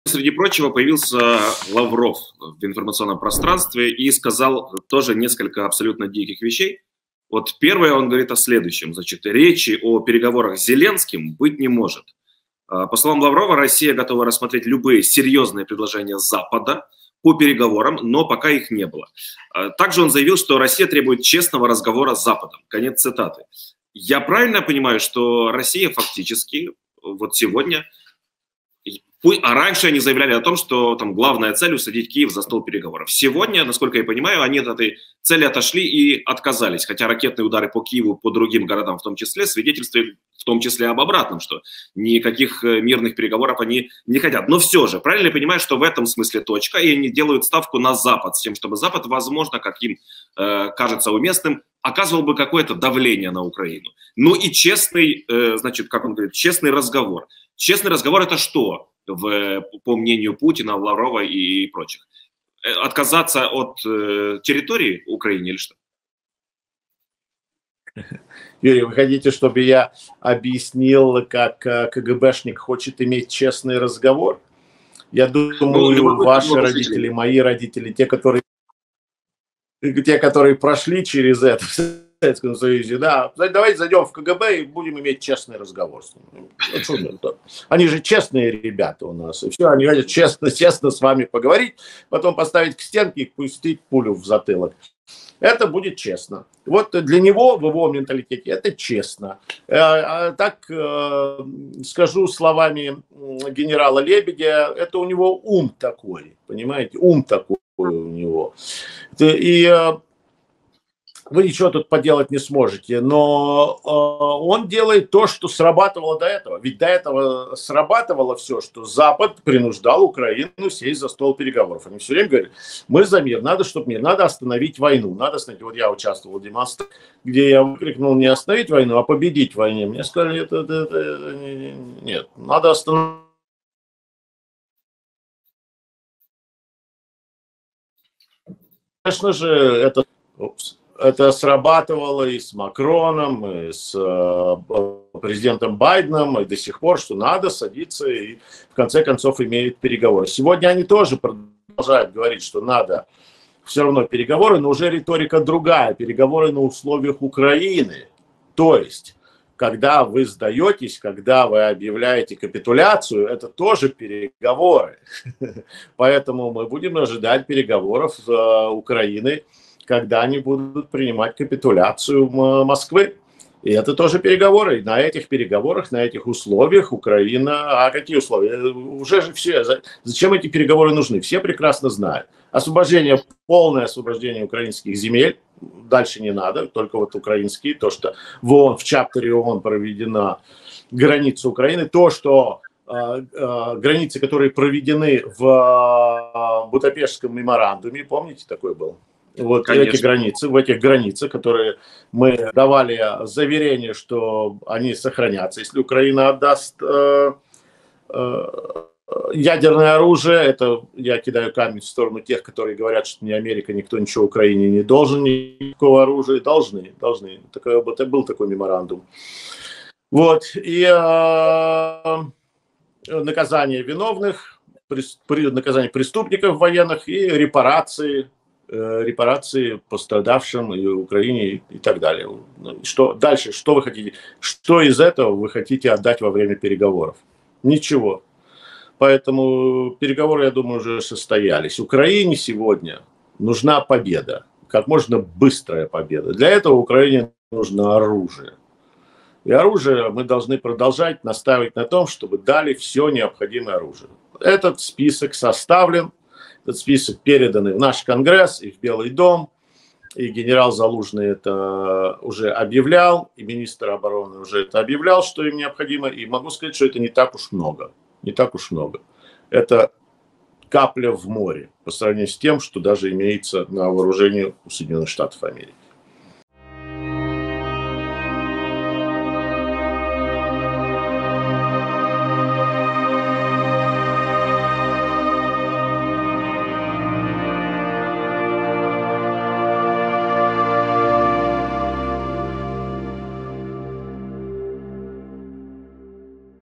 🔴ПЕРЕГОВОРИ БУДУТЬ, АЛЕ Є НЮАНС! Табах: Росія зробила СТАВКУ, ЯКА ВЖЕ РАЗ СПРАЦЮВАЛА
Гаррі Табах, капітан першого рангу ВМС США, в інтервʼю з Юрієм Бібіком спеціально для ютуб-каналу «Фабрика новин» розповів, що Росія знову взялася за своє і вимагає, щоб Захід натиснув на Україну щодо перемовин. Однак не все так просто, каже гість. Усі деталі дивіться в цьому відео!